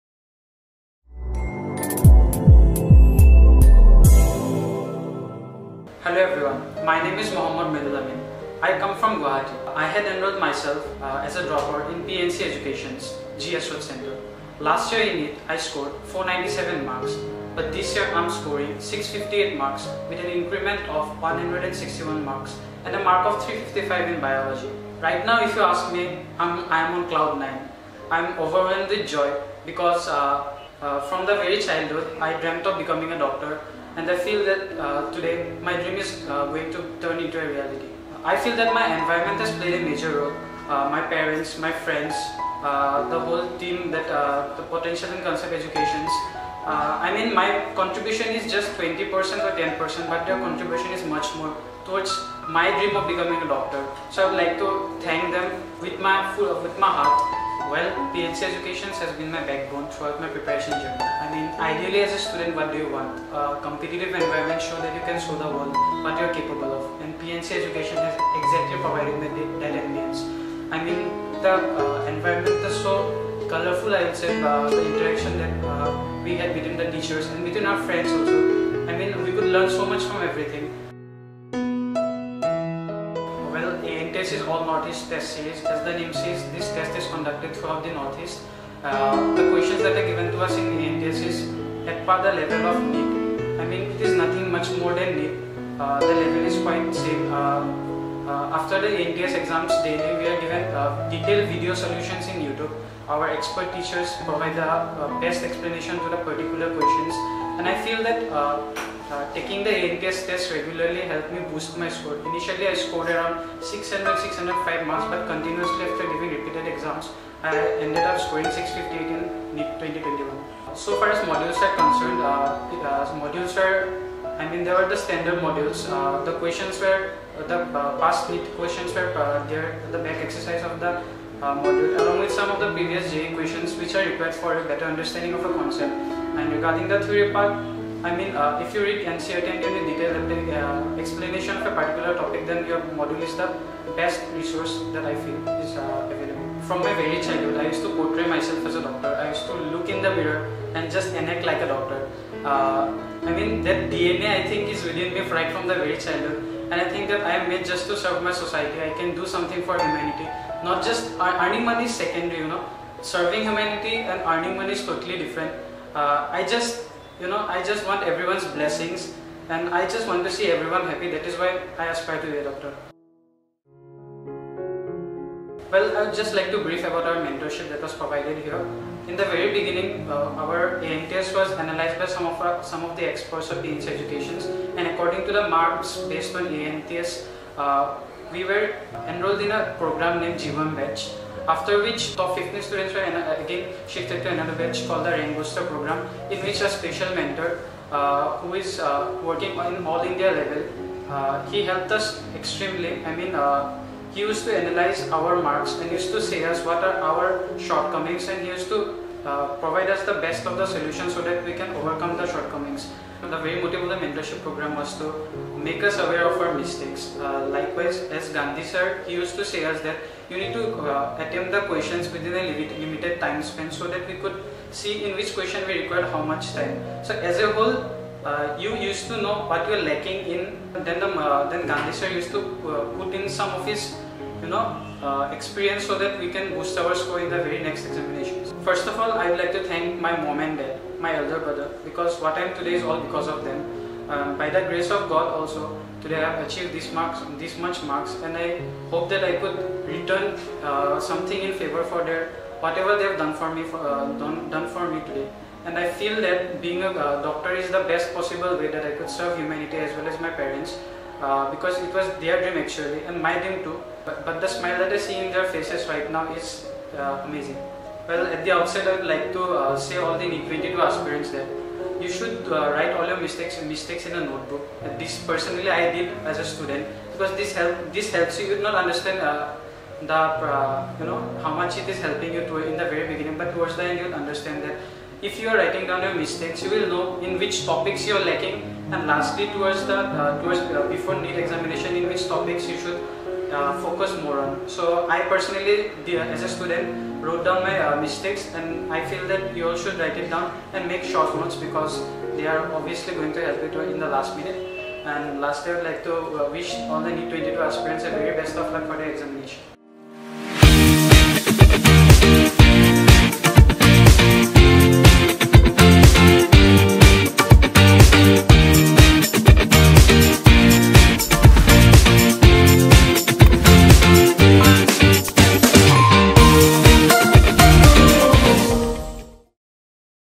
MD Mridul Amin | 658 Marks in NEET 2021 | Potential & Concept Educations
Hearty Congratulations to Mridul Amin for scoring 658 Marks in NEET 2021. We wish him all the success in life !!

After a grand success and celebration for producing the best results in NEET 2021 among all the institutes of the Northeast, we asked our students to share their experiences and some genuine reviews from them for all the guidance and mentorship they have received at Potential & Concept Educations. And they very willingly and happily shared their experiences
Here is what our Topper, Md. Mridul Amin has to say about his journey and experiences in our institute.